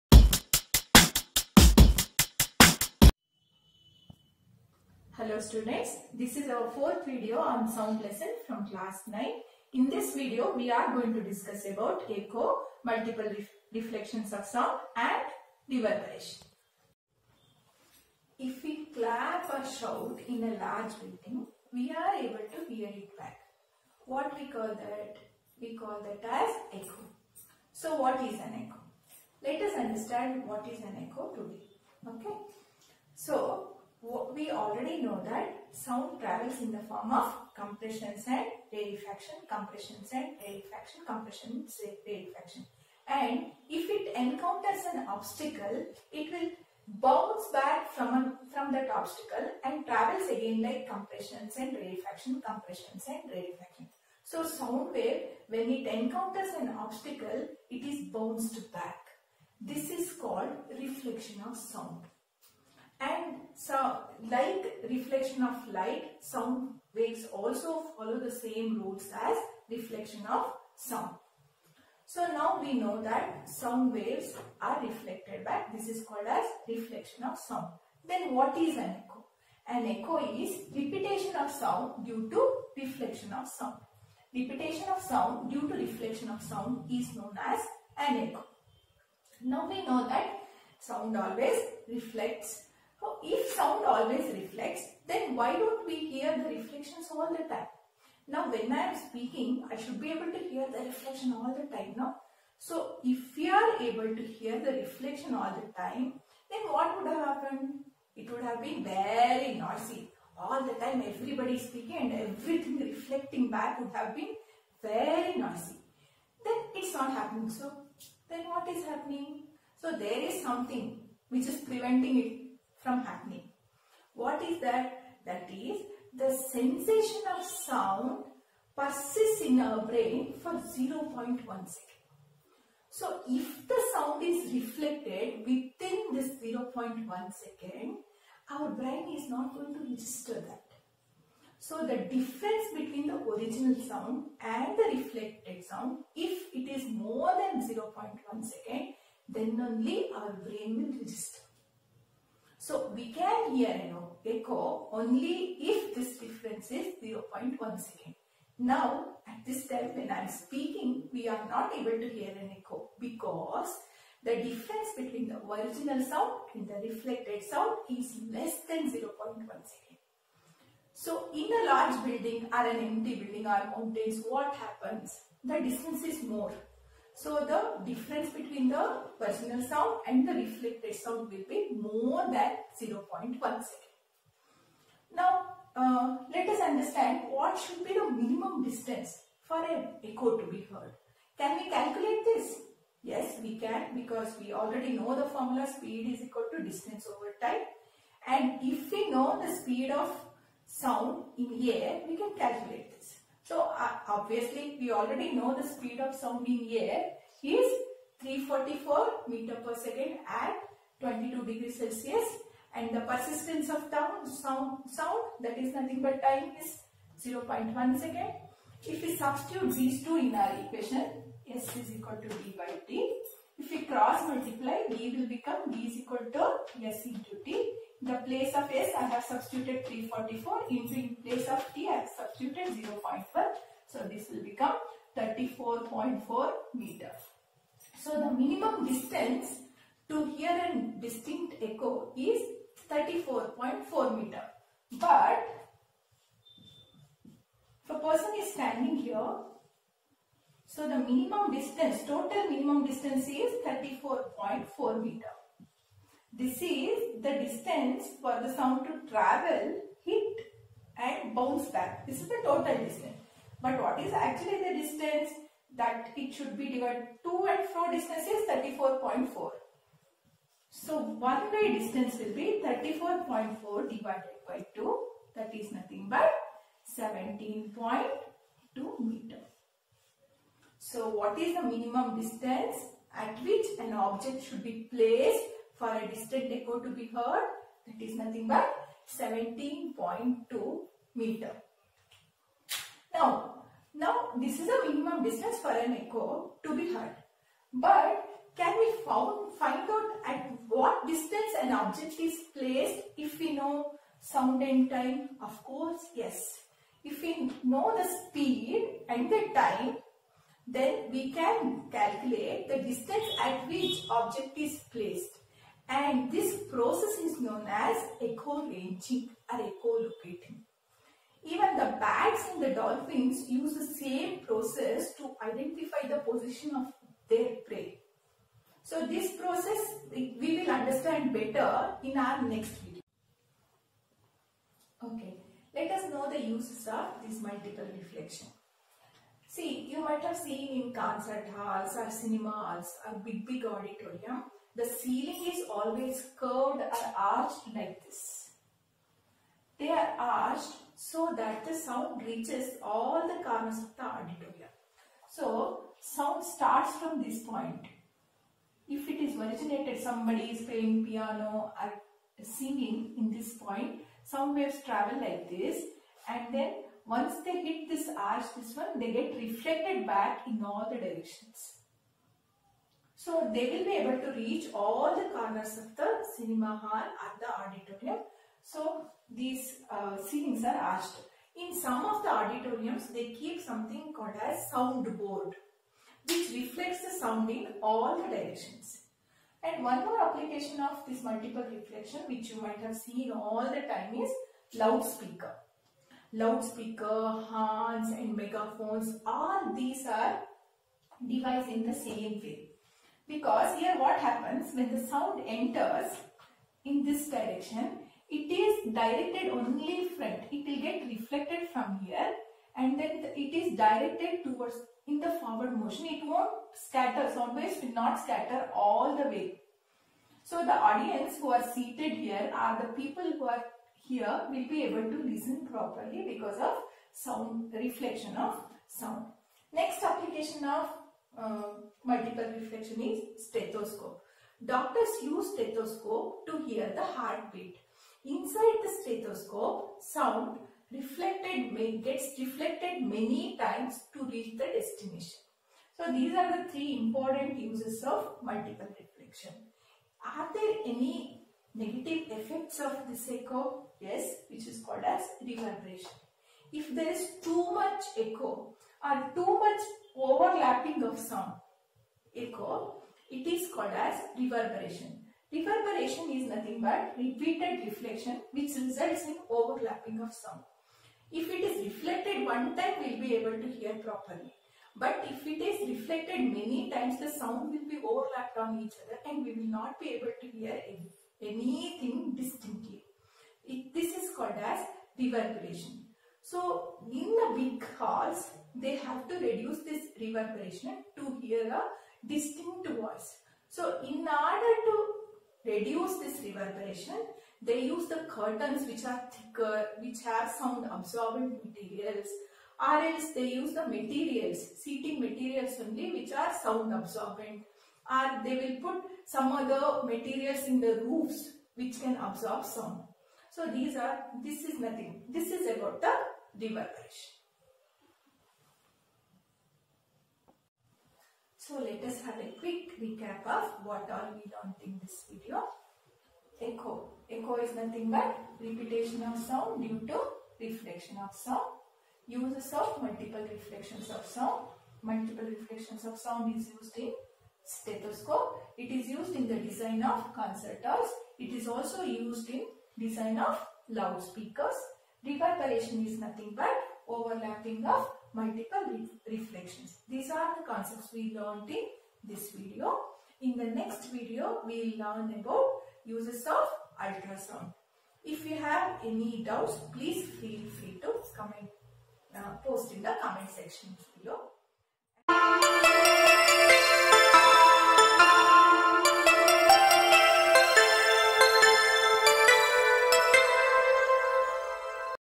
Class 9 Sound 4: Echo And Multiple Reflections Of Sound
Sound: Class 9 Science (Physics)
Echo: Repetition of sound due to reflection of sound
Uses of multiple reflection of sound: Stethoscope, Concert hall and Speaker
Reverberation: Overlapping of multiple reflections

VIGNYAN GURU IS NOW NJOY PHY